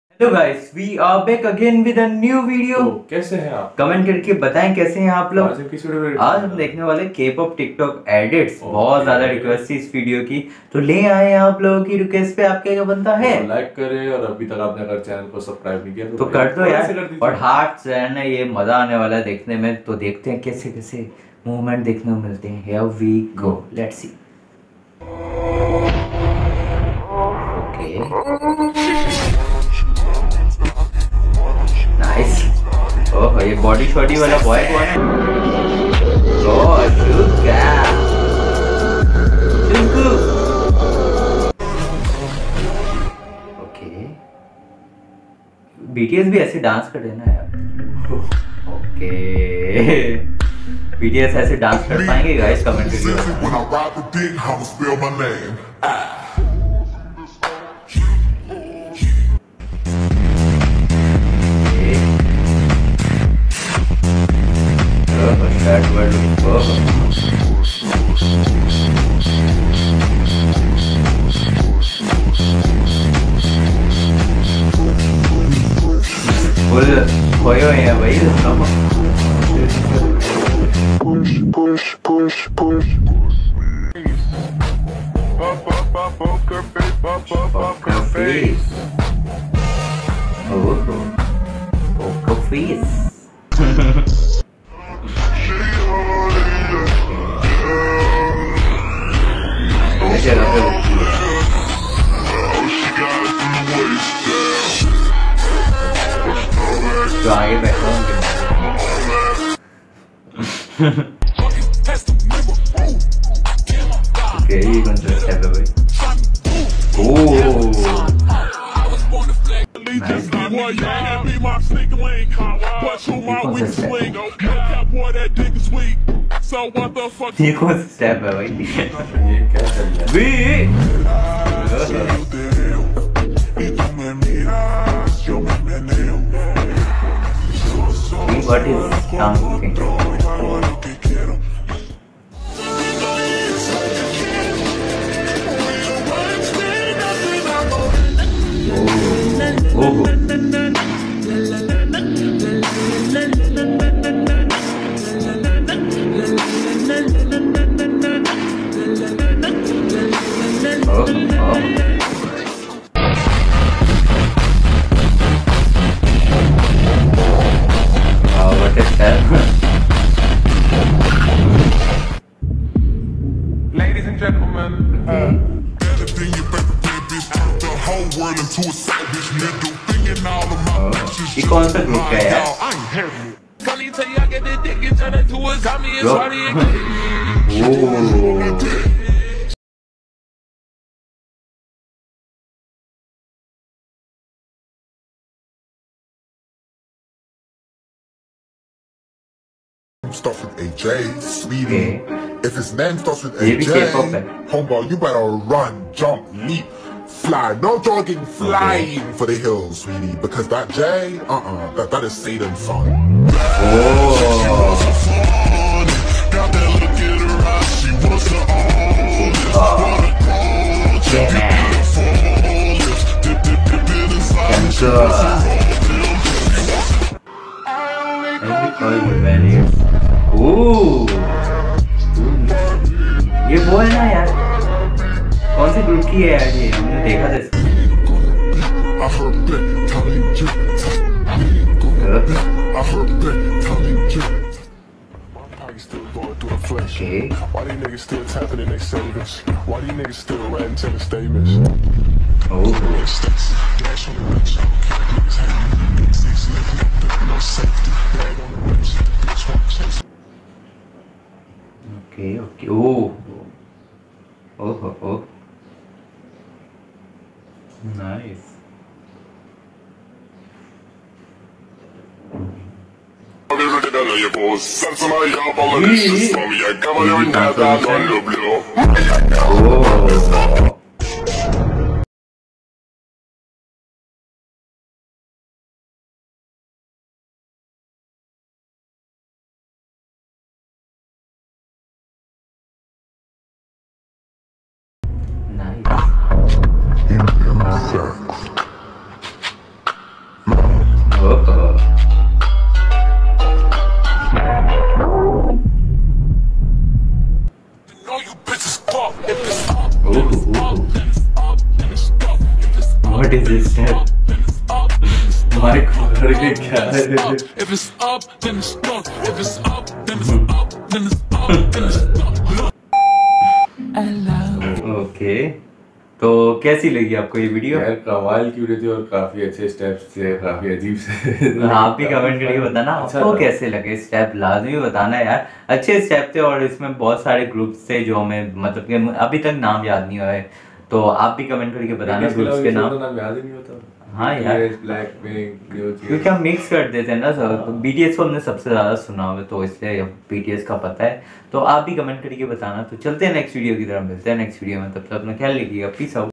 Pakistani reacts to Kpop tiktok edits | BTS | BLACKPINK
itxasad.official